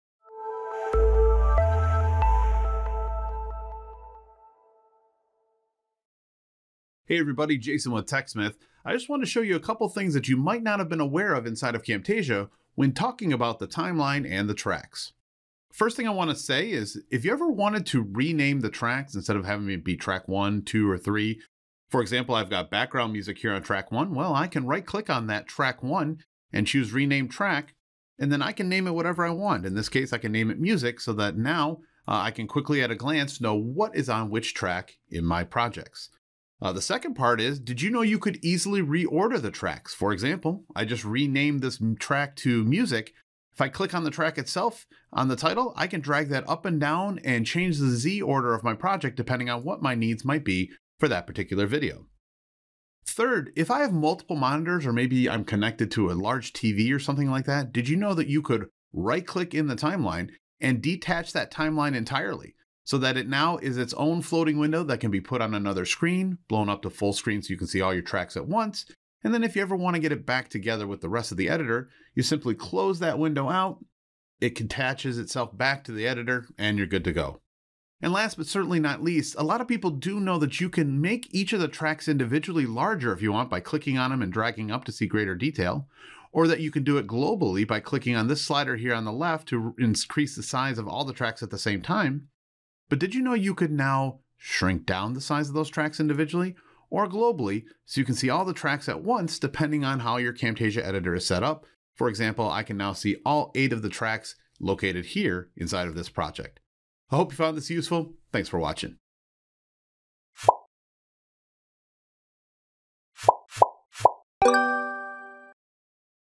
4 THINGS YOU MIGHT NOT KNOW ABOUT CAMTASIA TRACKS - TechSmith Tidbits
As we conduct more and more webinars we try to collect items of note that could be helpful hints for our users.

Jason had a small list of those items here. Possibly little know capabilities of the tracks and timeline in Camtasia.

Chapters:
0:16 Rename Tracks
0:35 Reorder Tracks
0:53 Detach the Timeline
1:18 Resize Tracks